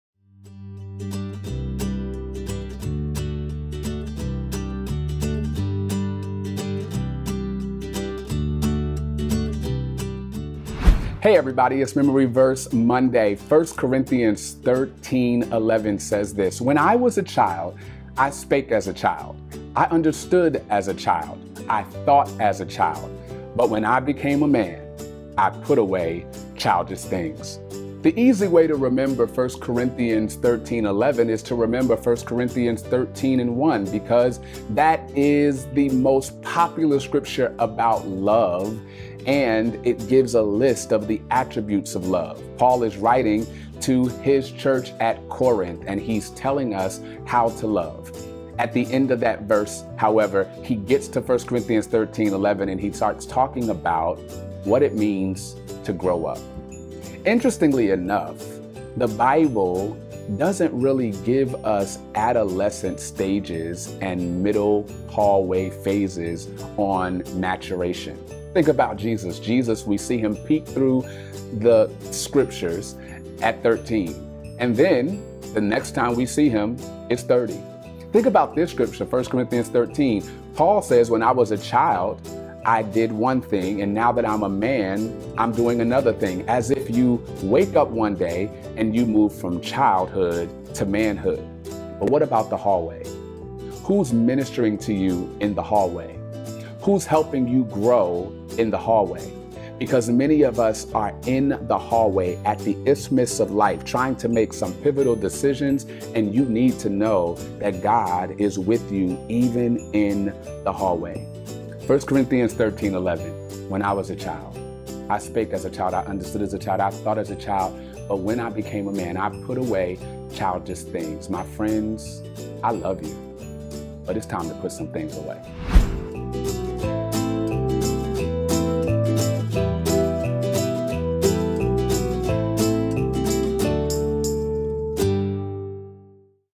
Memory Verse Monday - Shaun Saunders (1 Corinthians 13:11)
Happy Monday! And welcome to a time of focus, fruitfulness and memorization. MemoryVerseMonday reminds us of the beauty of Scripture. Today’s verse comes from the book of Corinthians. It’s a good one. God bless!
To receive weekly motivations directly to your phone or email, text SHAUN to 313131. It’s FREE!